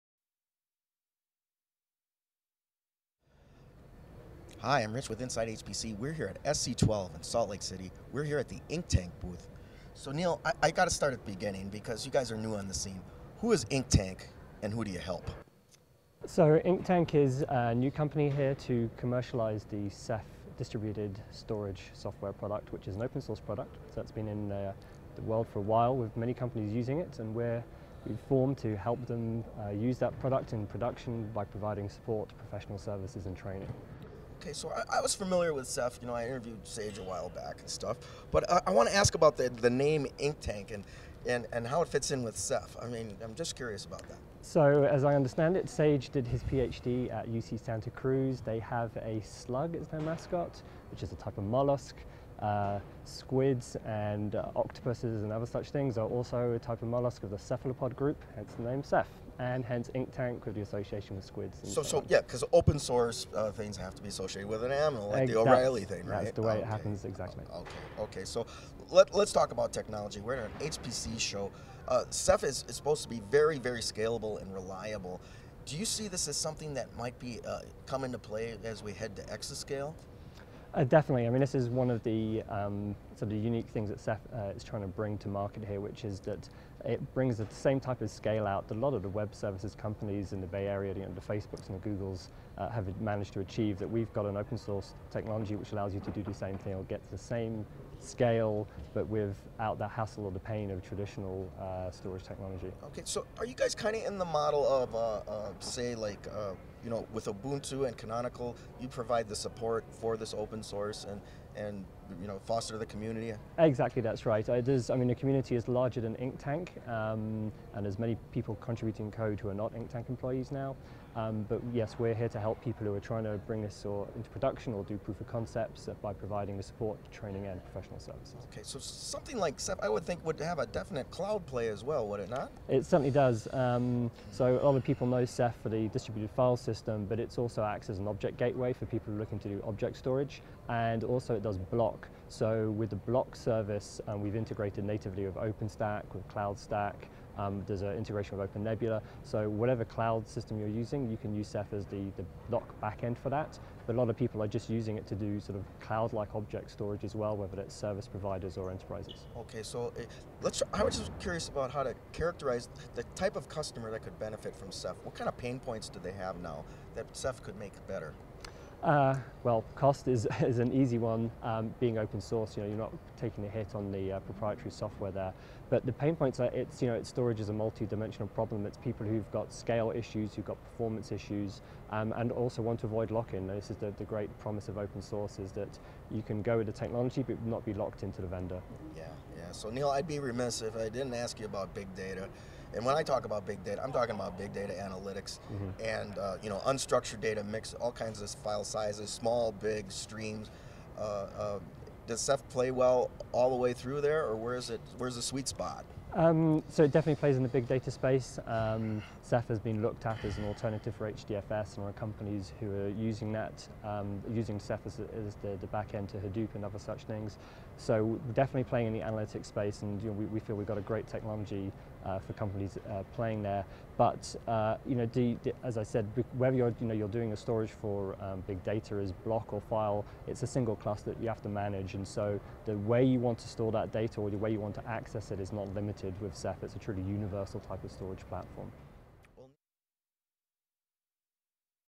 SC12 - Inktank Boosts Open Source Ceph File System for the Enterprise
Our VP of Product, Neil Levine speaking at SC12 in Nov 2012.